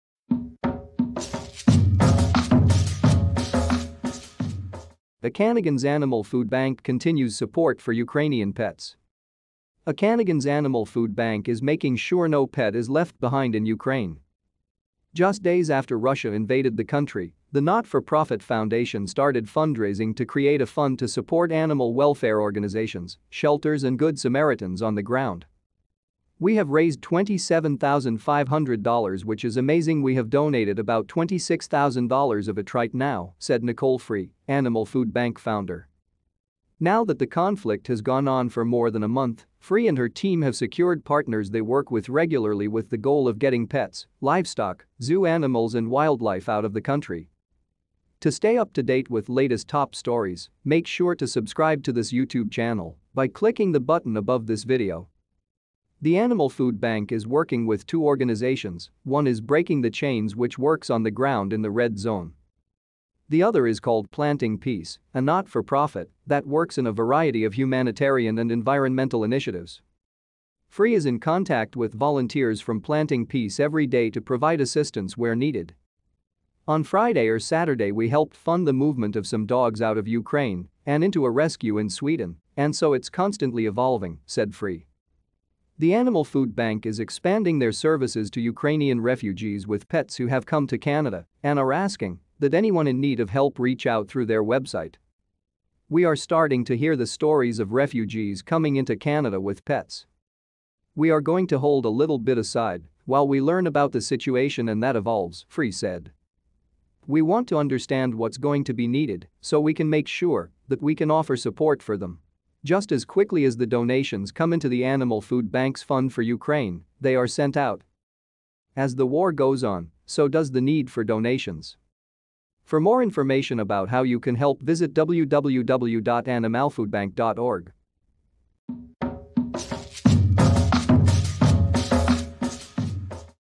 Okanagan’s Animal Food Bank continues support for Ukrainian pets
Okanagan’s Animal Food Bank is making sure no pet is left behind in Ukraine.
Just days after Russia invaded the country, the not-for-profit foundation started fundraising to create a fund to support animal welfare organizations, shelters and good Samaritans on the ground.
“We have raised $27,500 which is amazing we have donated about $26,000 of it right now,” said Nicole Frey, Animal Food Bank founder.
Now that the conflict has gone on for more than a month, Frey and her team have secured partners they work with regularly with the goal of getting pets, livestock, zoo animals and wildlife out of the country.
The Animal Food Bank is working with two organizations, one is Breaking the Chains which works on the ground in the red zone.
The other is called Planting Peace, a not-for-profit that works in a variety of humanitarian and environmental initiatives.